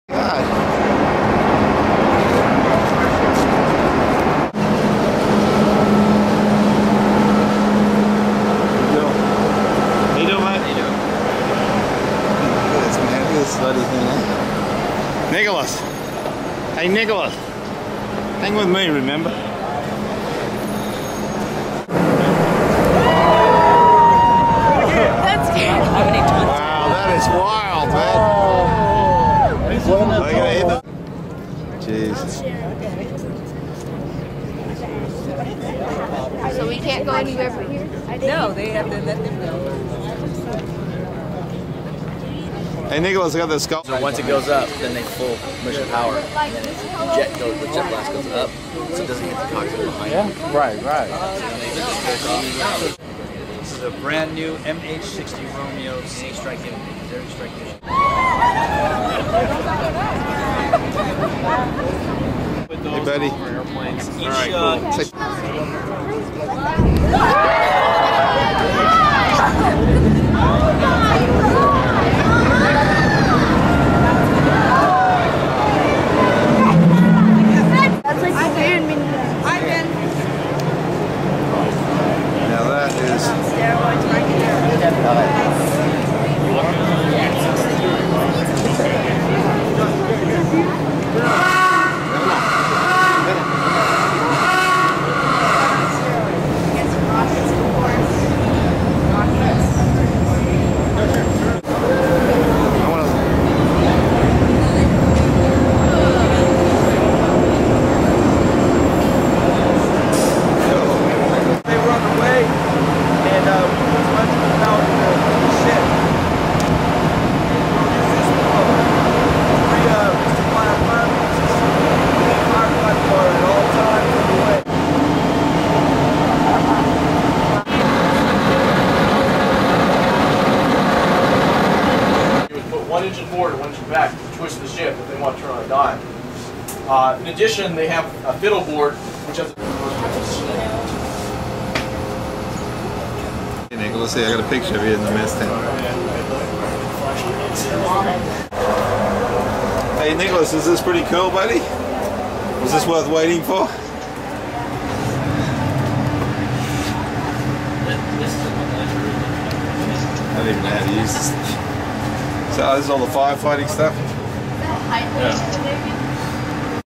Navy Week
Nicholas at Navy week in Los Angeles Harbor 2011